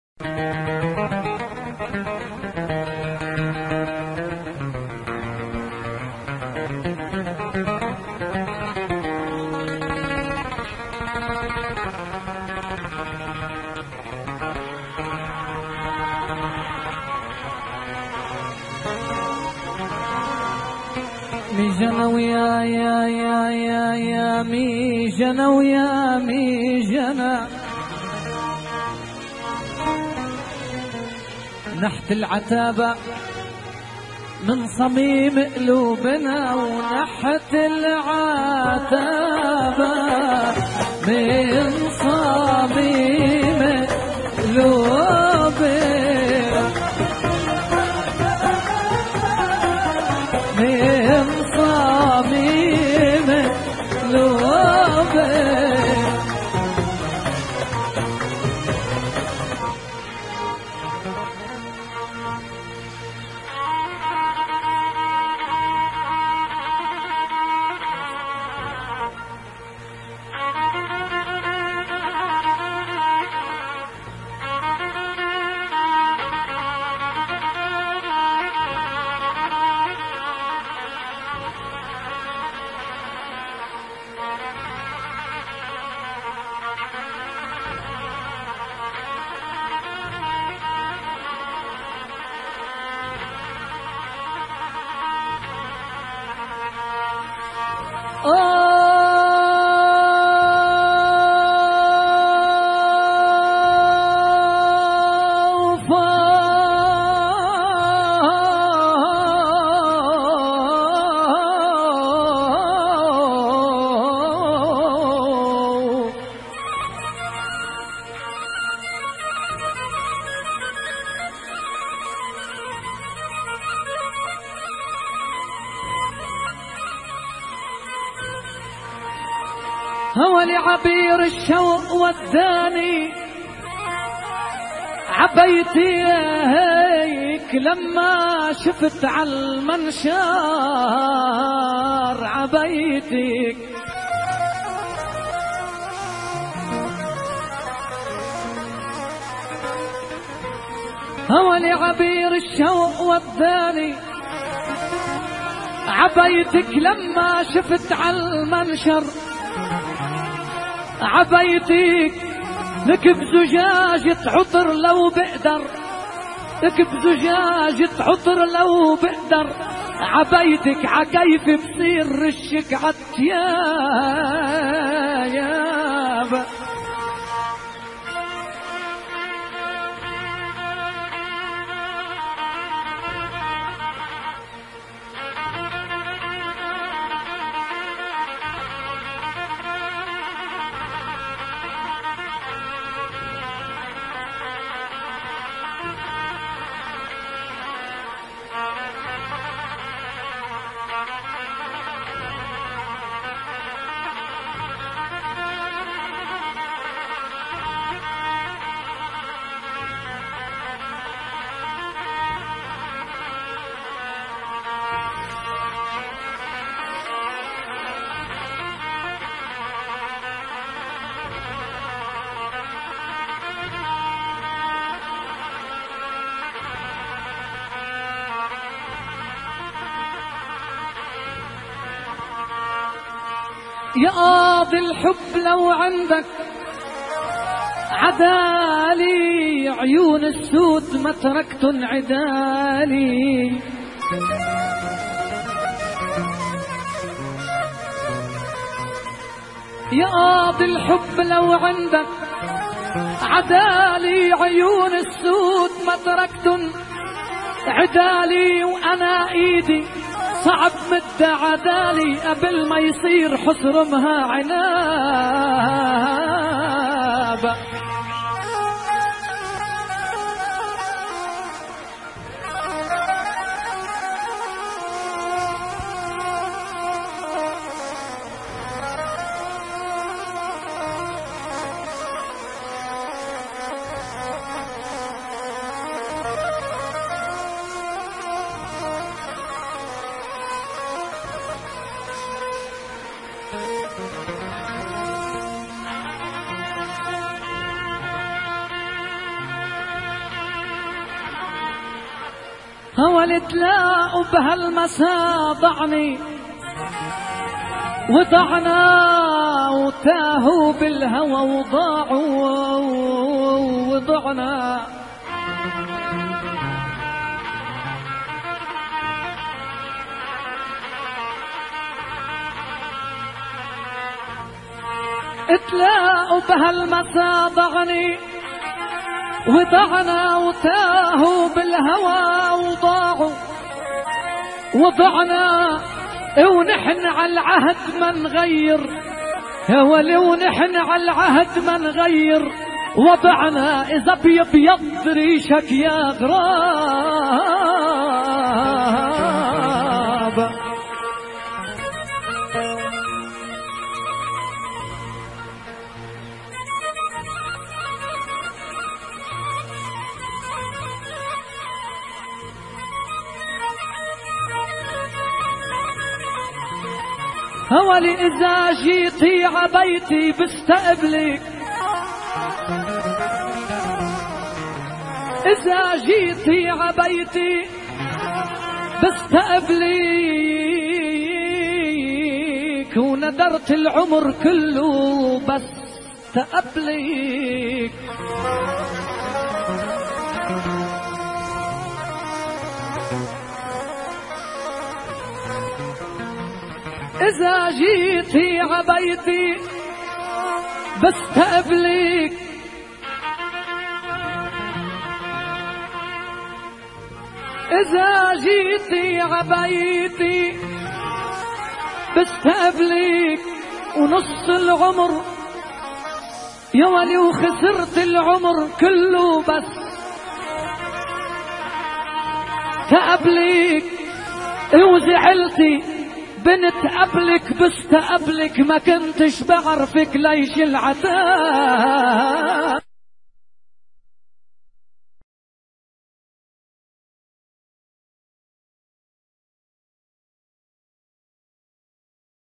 walid sarkis mijana
walid sarkis singing mijana and aataba